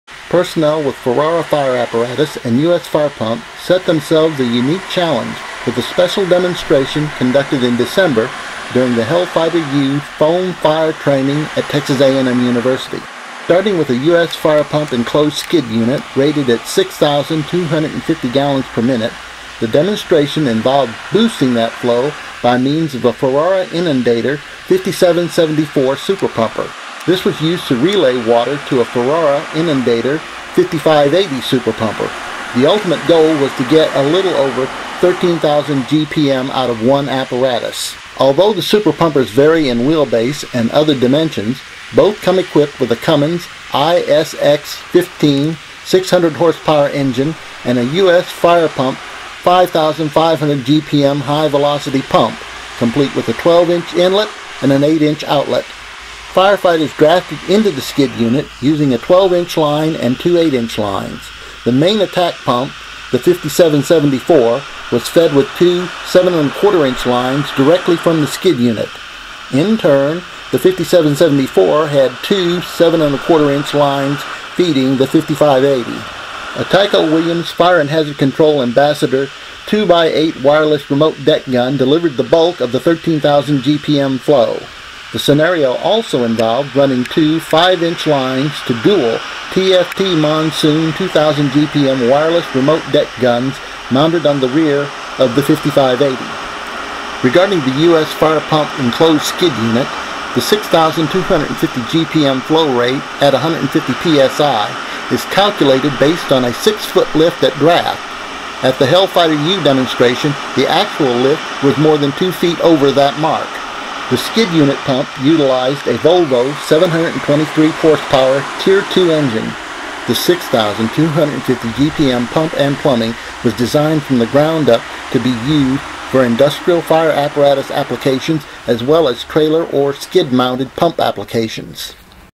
Ferrara - U.S. Fire Pump Flow Test
A flow test conducted during the Hellfighter U fire training in December 2015 involved a US Fire Pump skid unit rated at 6,250 gpm, boosting that flow using two Ferrara Inundator Super Pumpers. The final goal was 13,000 gpm.